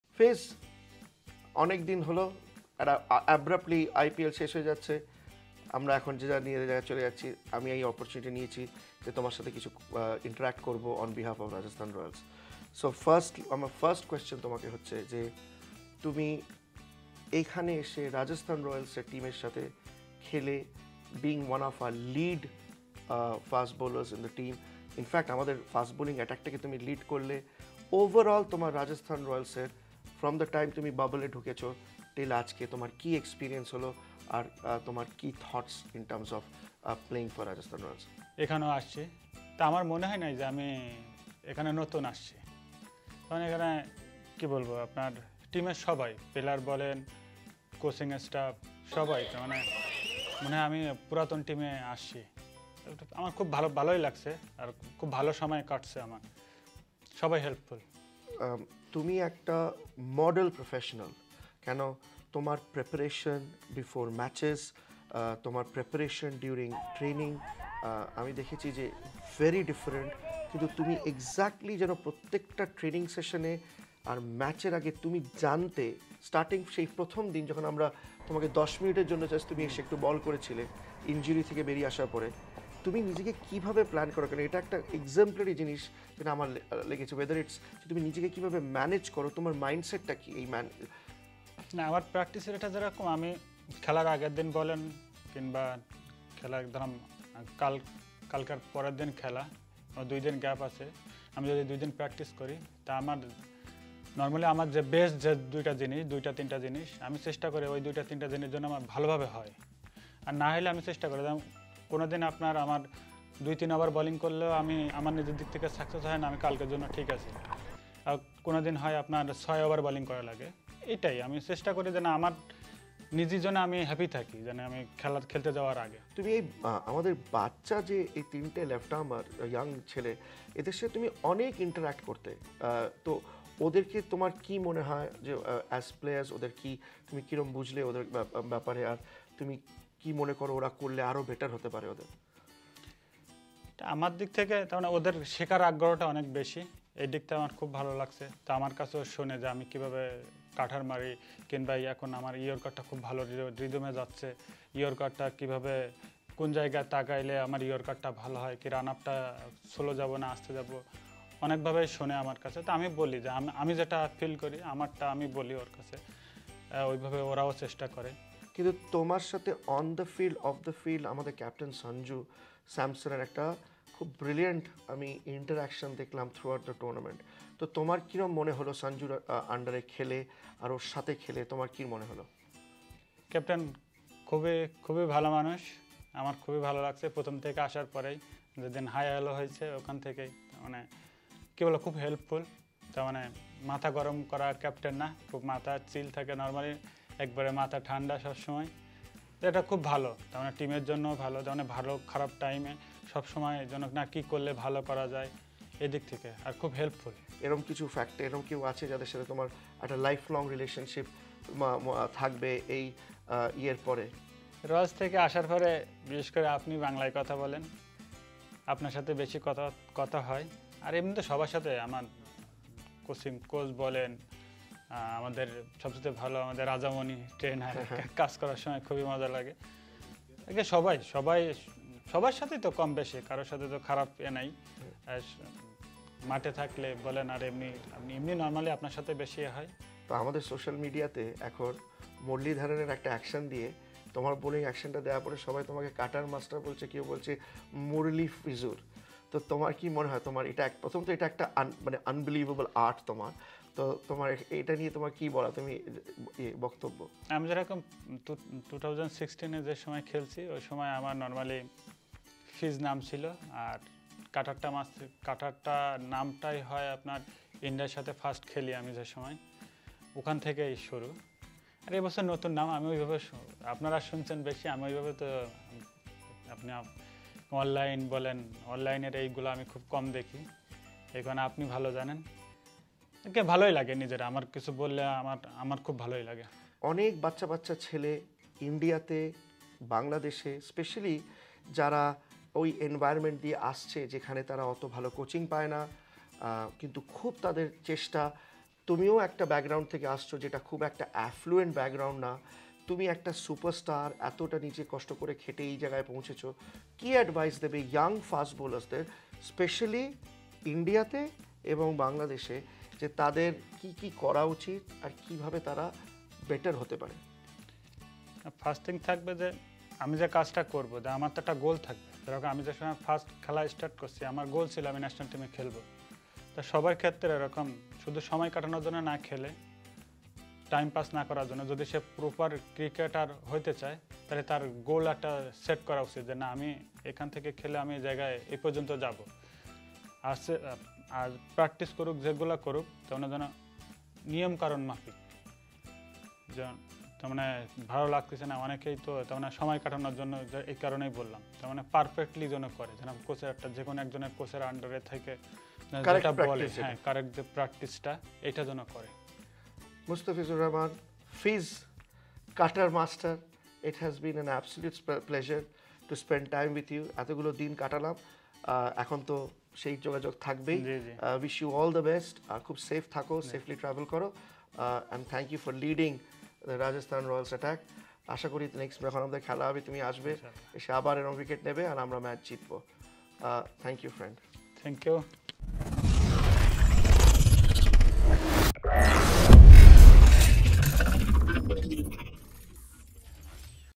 EXCLUSIVE! Cutter Master on playing with Rajasthan Royals
Sanju's leadership 🙌
Guiding young pacers 👨‍🏫
Reaction to Murali-Fizur 🕸

Watch Mustafizur unwind in Bangla. 🎥